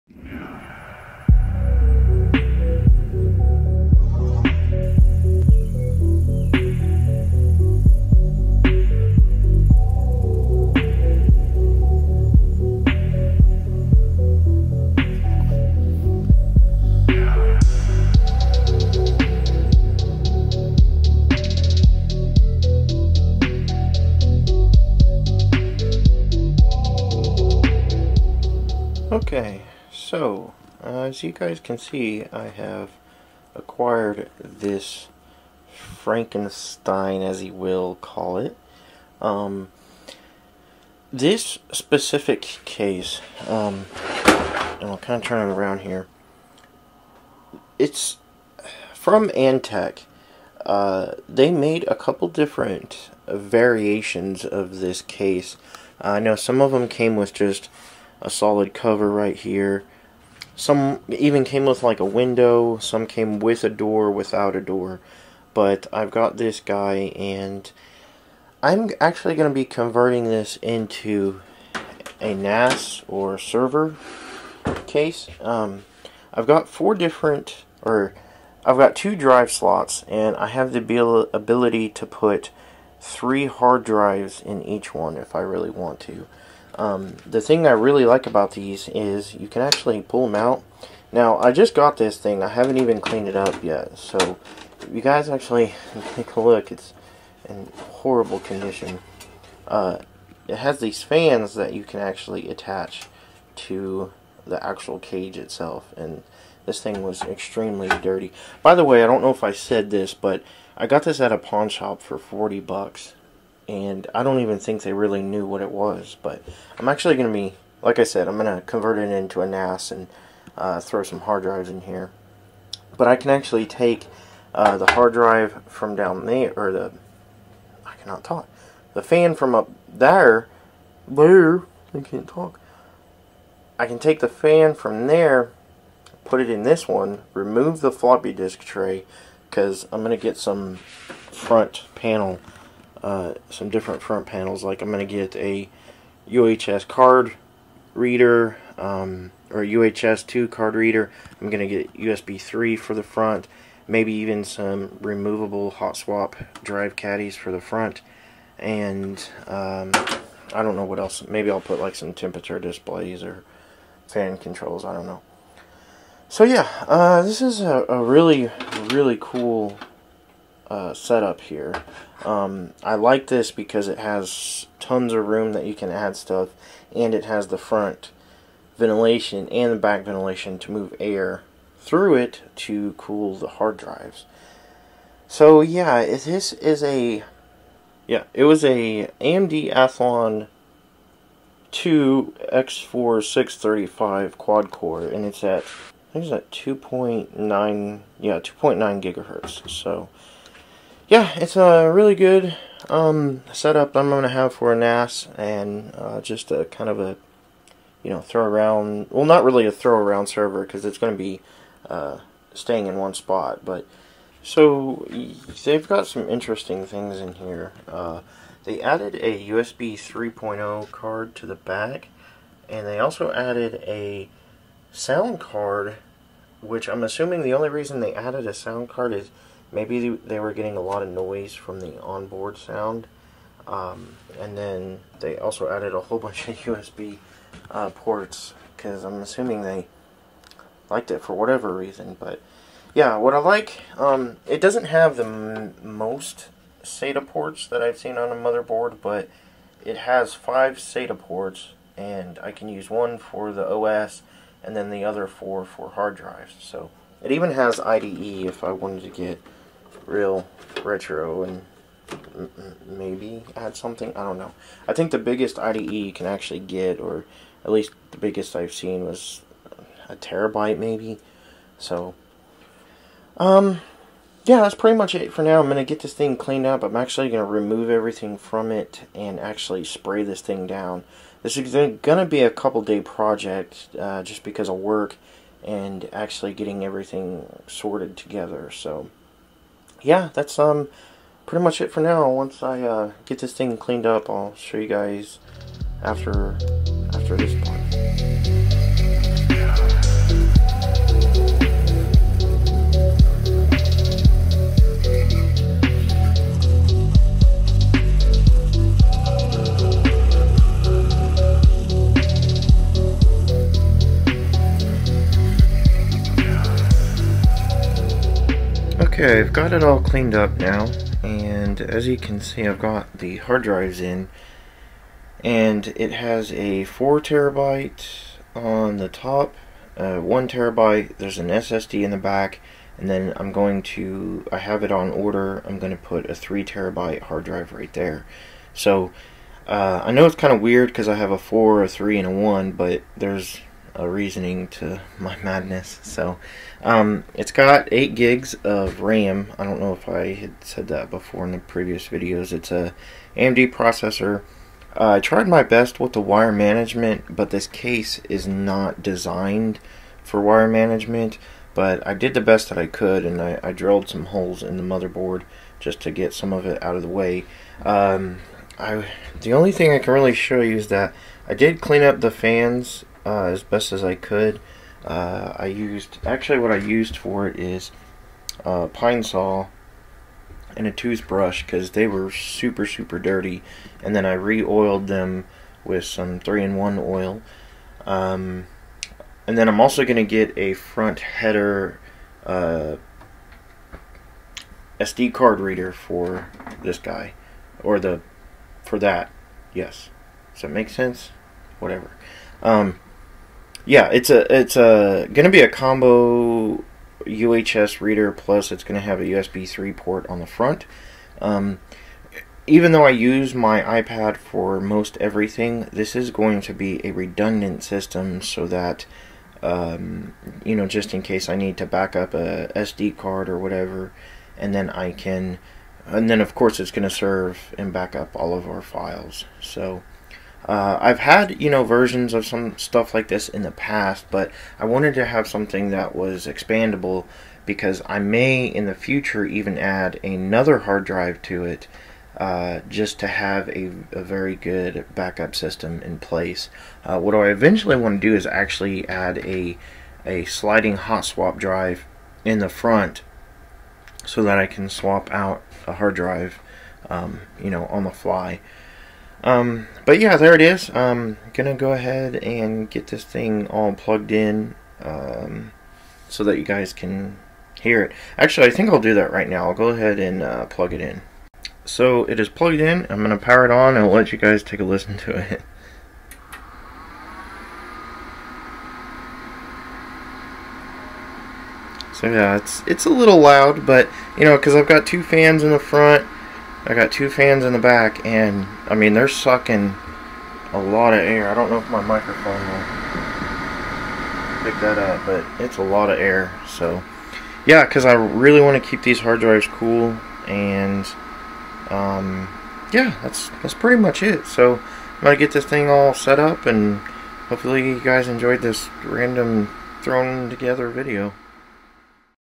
Cheap NAS Build, Antec/Cheiftec Case
Wasnt sure what to title this as but I build a NAS/Server for all my files, will update soon once all the kinks are irons out.